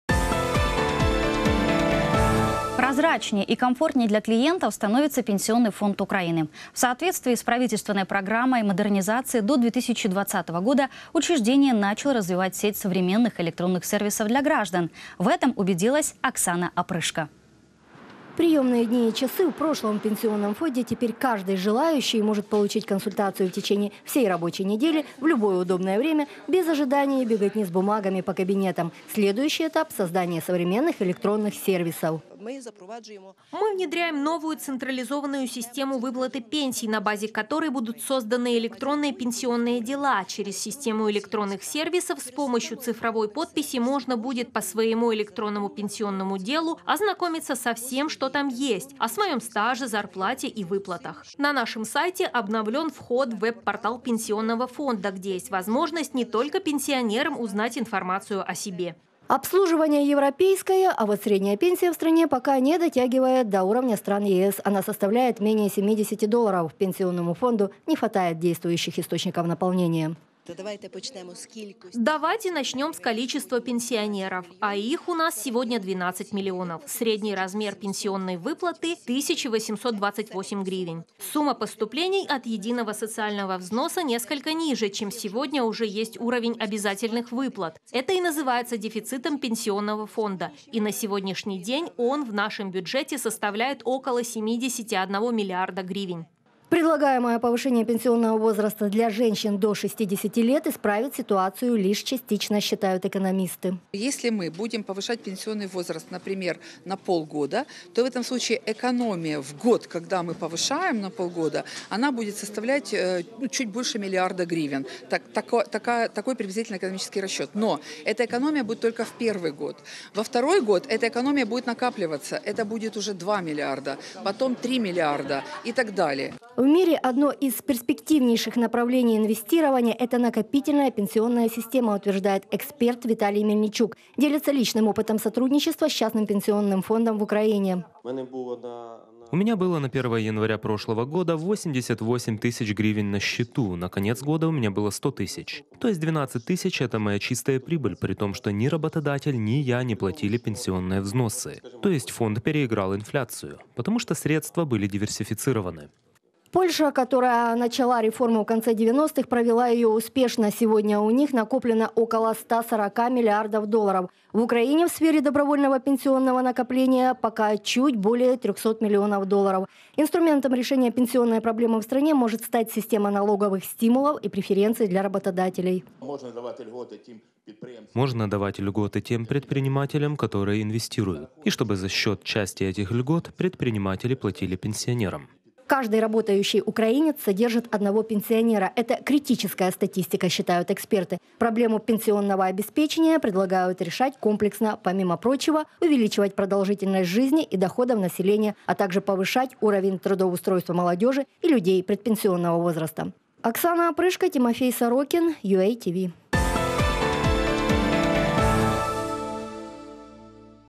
Пенсионный фонд Украины становится комфортнее для клиентов
Пенсионный фонд Украины
В соответствии с правительственной программой модернизации до 2020 года, учреждение начало развивать сеть современных электронных сервисов для граждан.